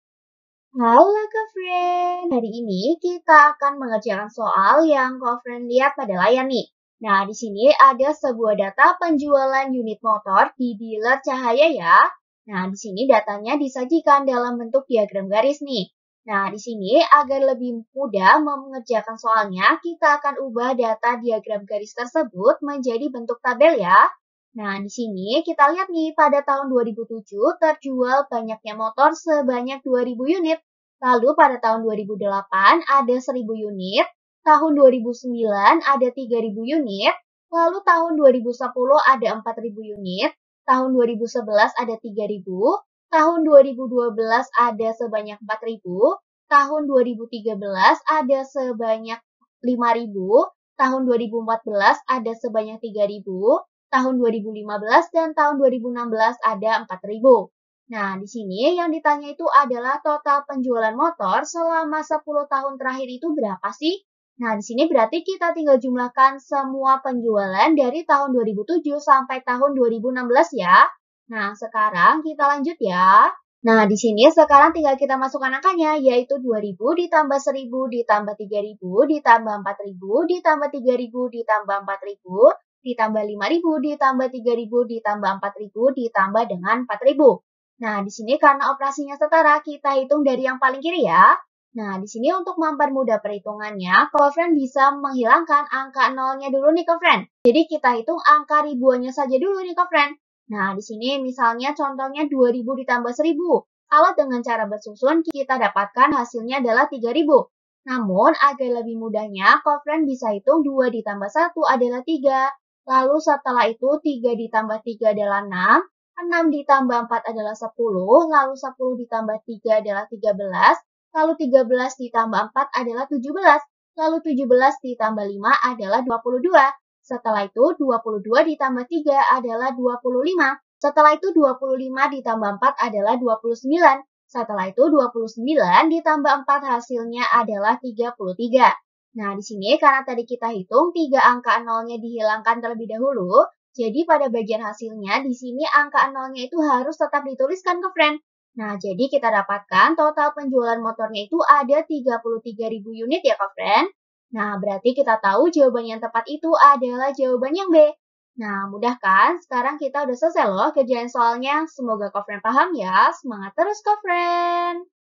Data penjualan unit motor di Dealer Cahaya tersaji dalam diagram garis berikut 1 5.000 4.000 { 3....
Data penjualan unit motor di Dealer Cahaya tersaji dalam diagram garis berikut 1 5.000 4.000 { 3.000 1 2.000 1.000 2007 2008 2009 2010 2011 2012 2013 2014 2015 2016 Tahun Total penjualan motor selama 10 tahun terakhir adalah sebanyak unit; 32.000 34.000 a. C. 33.000 d. 35.000 b

Topik atau Materi: Penyajian Data dalam Bentuk Diagram Garis - Pengumpulan dan Penyajian Data - Statistika - Matematika Kelas 5 SD - K-13 Revisi

Sumber Soal: Matematika Kelas 6, K-13 Revisi, RS, 2021 , Masmedia

00:00 : Bedah Soal
01:00 : Konsep, rumus dan pengertian Penyajian Data dalam Bentuk Diagram Garis
01:18 : Langkah penyelesaian soal
01:45 : Jawaban akhir
02:15 : Penutup